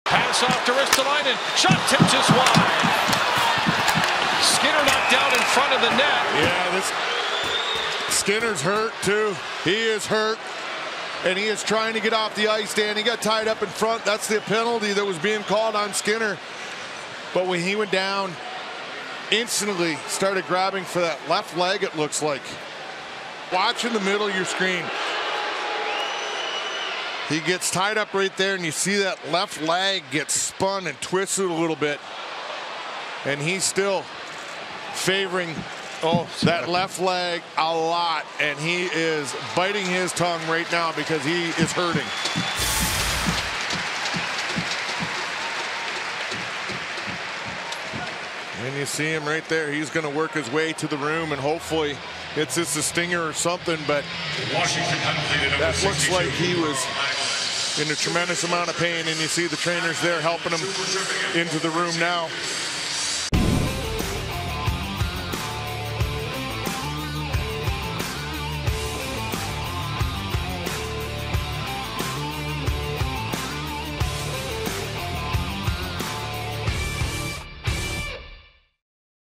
Jeff Skinner Helped Off Ice After Carl Hagelin's Stick Twists His Leg
Jeff Skinner got tripped by Carl Hagelin and twisted his leg in the process, causing him to writhe in pain and require help to get off the ice.